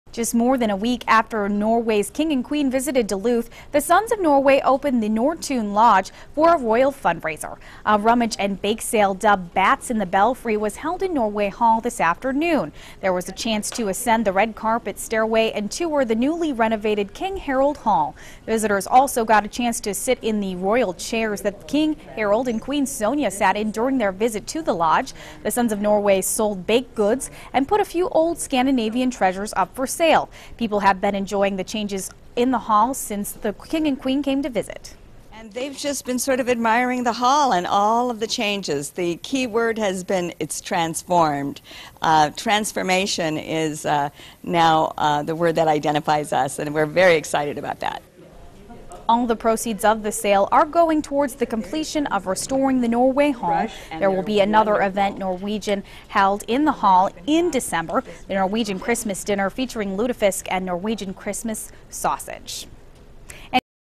Sons of Norway Garage Sale in Duluth Minnesota
Not long after the Norwegian royalty visited Duluth, the Sons of Norway opened the Nortun Lodge for a royal fundraiser.

A rummage and bake sale dubbed "Bats in the Belfry" was held in Norway Hall Saturday afternoon. There was a chance to ascend the red carpet stairway and tour the newly renovated King Harold Hall. Visitors also got a chance to sit in the royal chairs that King Harold and Queen Sonja sat in during their visit to the lodge. The Sons of Norway sold baked goods and put a few old Scandinavian treasures up for sale.

All the proceeds of the sale are going towards the completion of restoring the Norway Hall. There will be another event held in the hall this december. A Norwegian Christmas Dinner featuring lutefisk and Norwegian Christmas sausage.